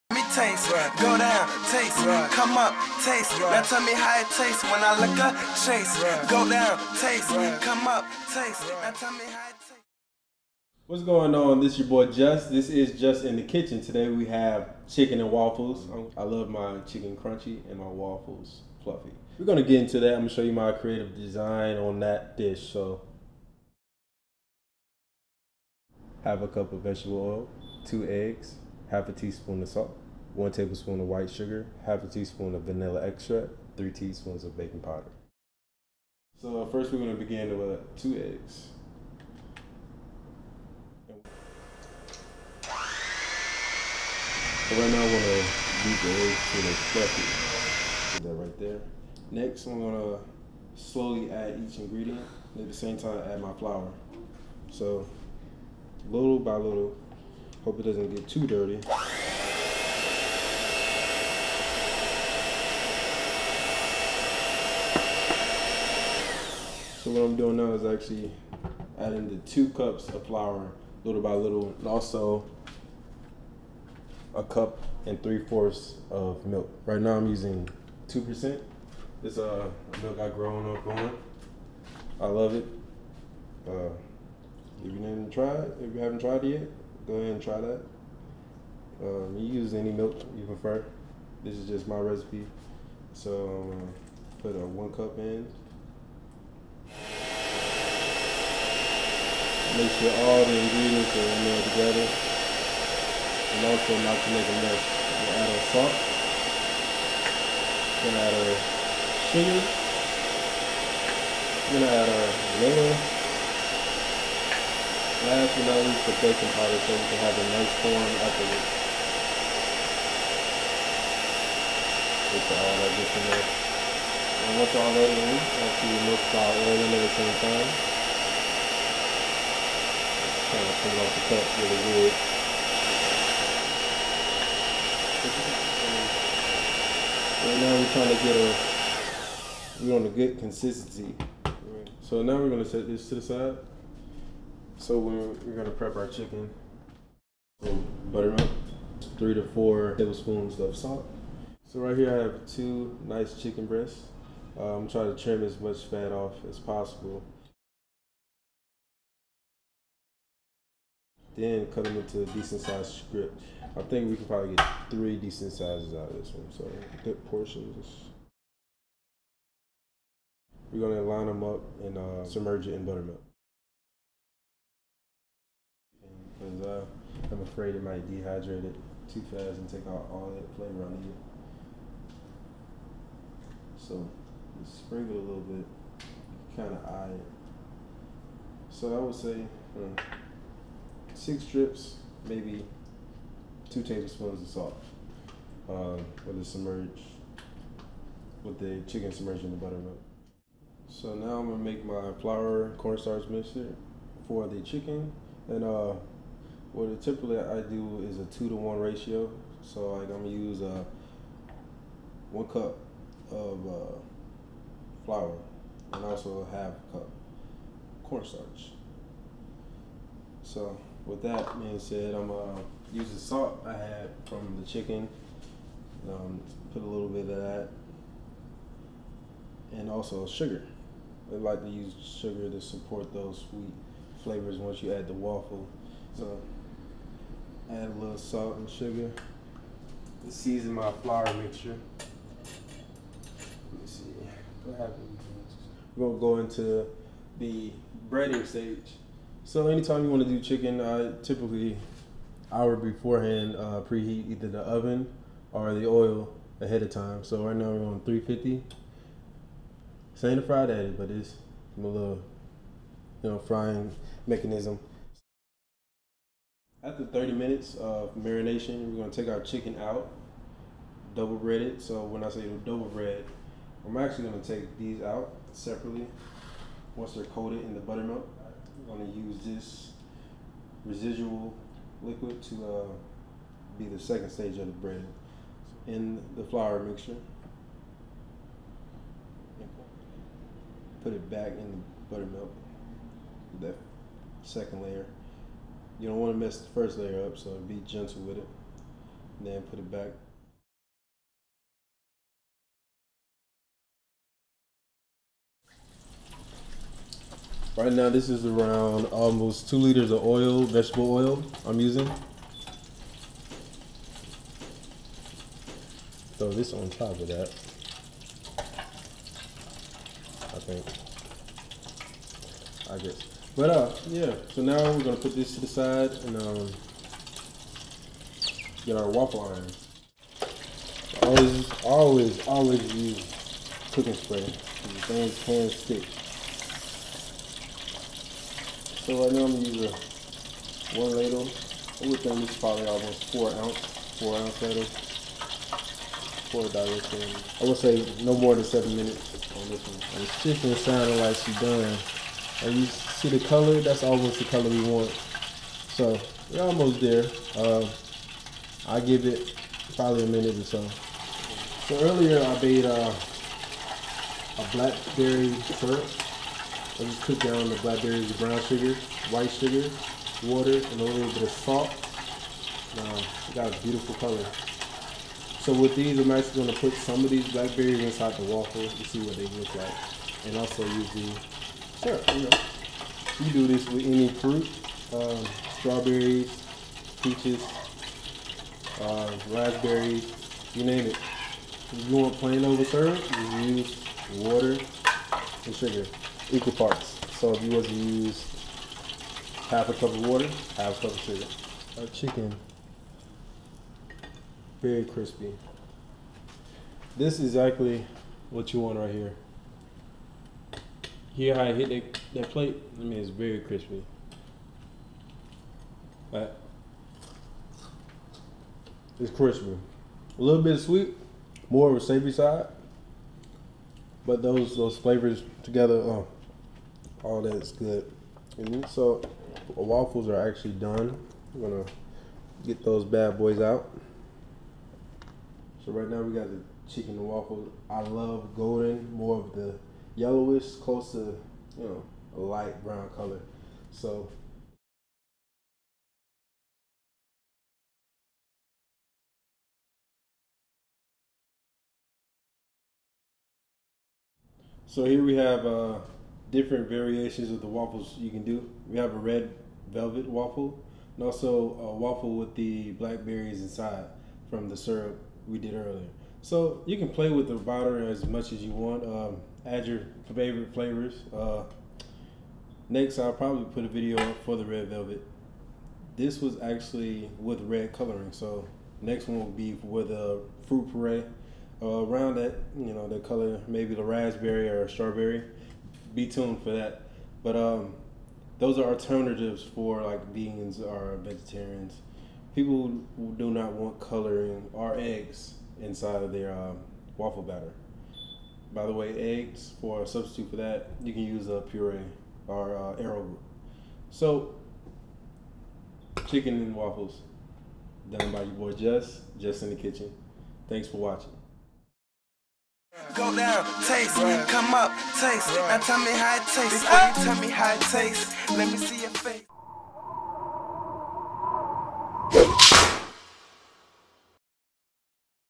Jess In The Kitchen (Shot By: Leelwan)
In the kitchen with Jess. He cooks up a delicious batch of Chicken & Waffles.

Chef - @sayyid_music